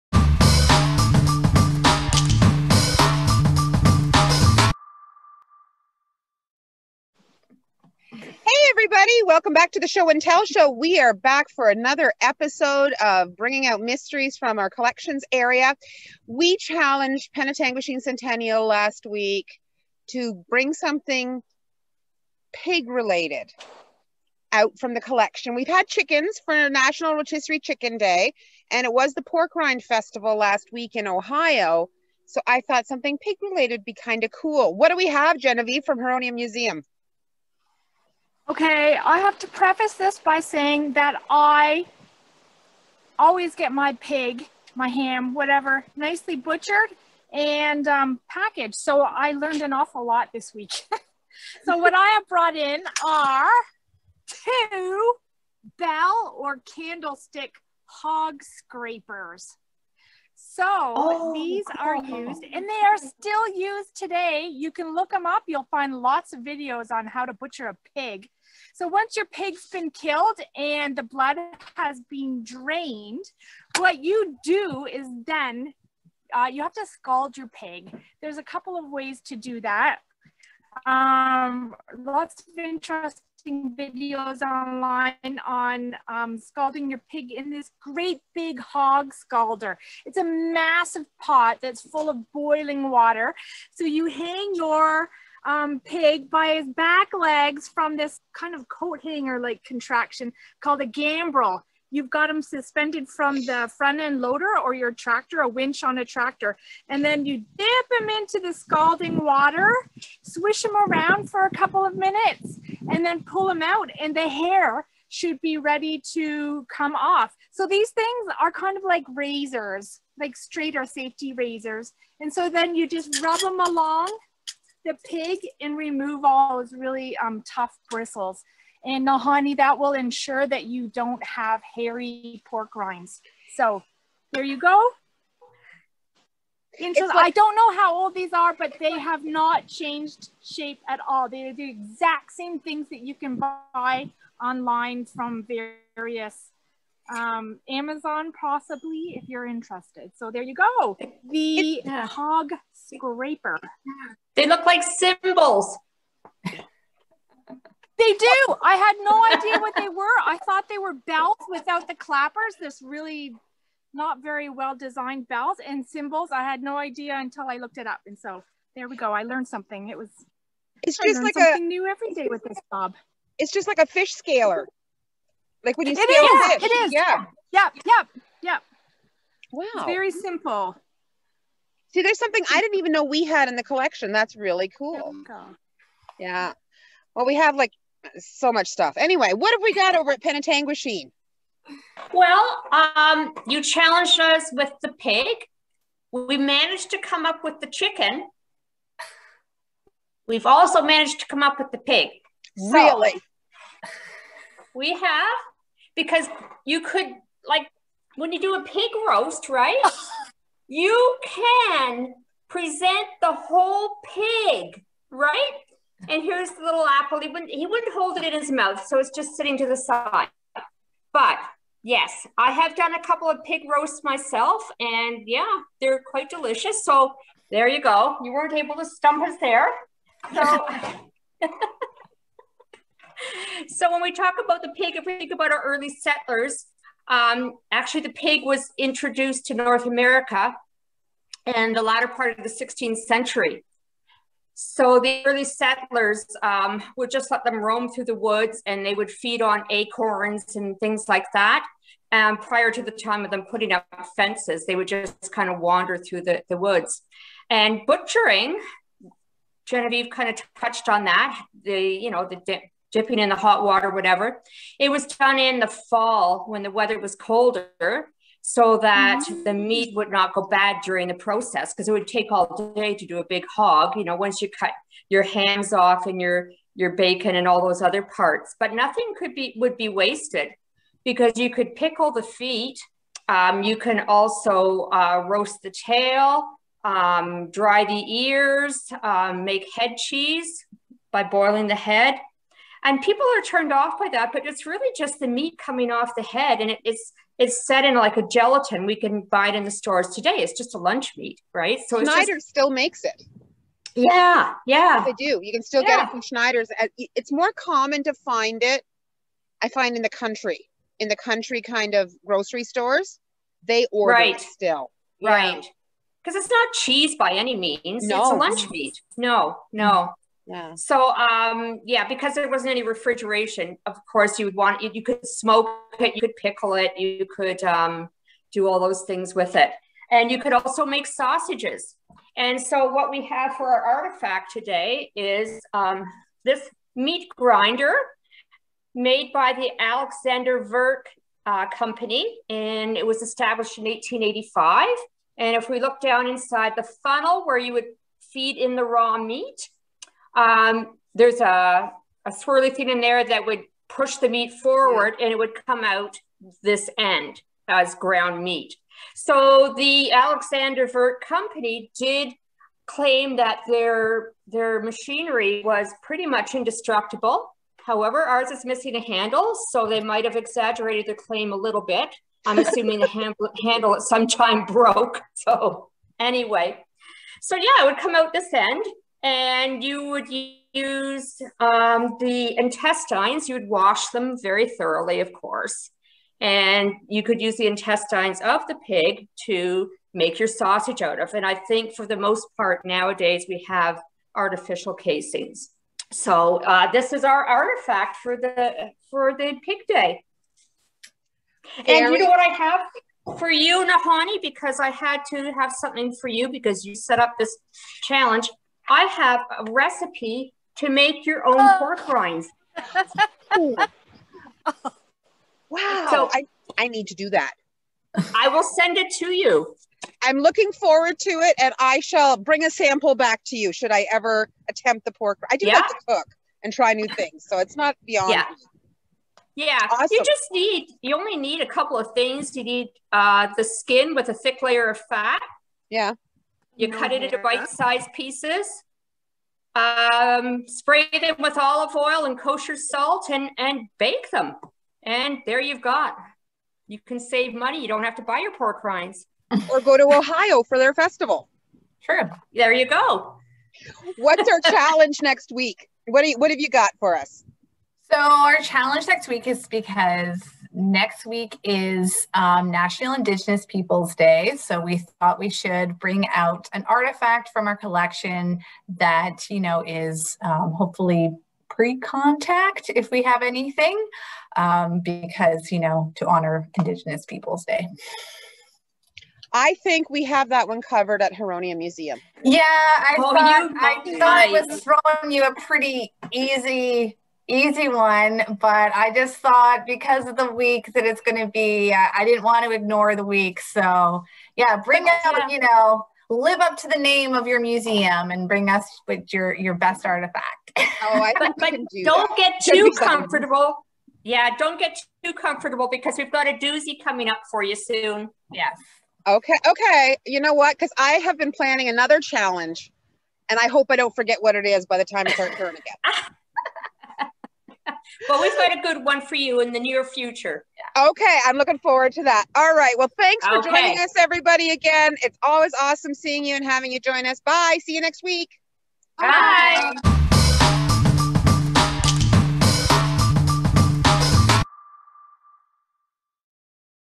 Show & Tell Show #30: Piggy Things
In today's episode of the Museum Show & Tell Show, Huronia Museum challenges Penetanguishene Centennial Museum & Archives to find a pig related artifact in their collection.

FYI, here is the recipe that Jan found for Nahanni for homemade pork rinds.

Homemade Pork Rinds

Start by cutting the pork skin into bite-size pieces.
If the skin has a thick layer of fat attached to it, trim it, but don’t remove it completely.
Arrange the pieces on a parchment-lined rimmed baking sheet, skin side up.  Spray them with olive oil and sprinkle them with kosher salt.  Be liberal with both the oil and the salt.  I start with 1 teaspoon of kosher and work your way up to personal taste.
Bake at 325 degrees until golden and crispy, probably 1.5 to 2 hours.
Let cool completely by laying them on paper towel, skin side up.  They will continue to crisp as they cool.